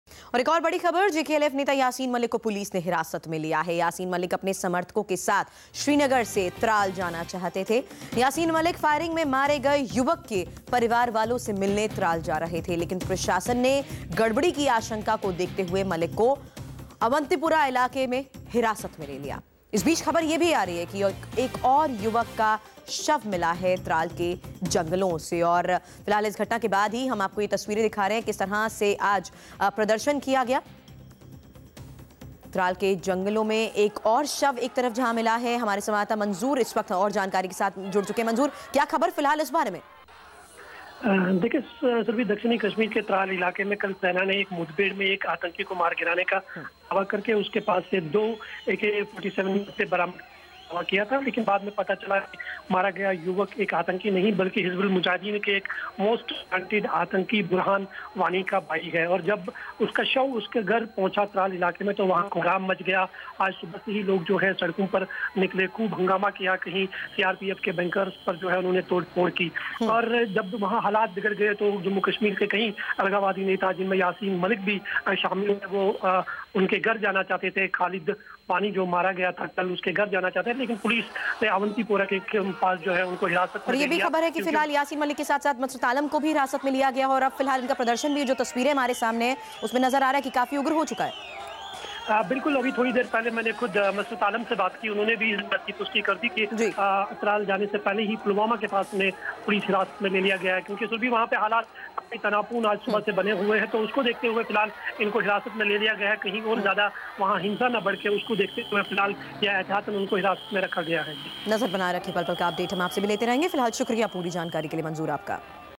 Separatist leaders Yasin Malik, Masarat Alam Bhat detained - India TV
Separatist leaders Yasin Malik, Masarat Alam Bhat detained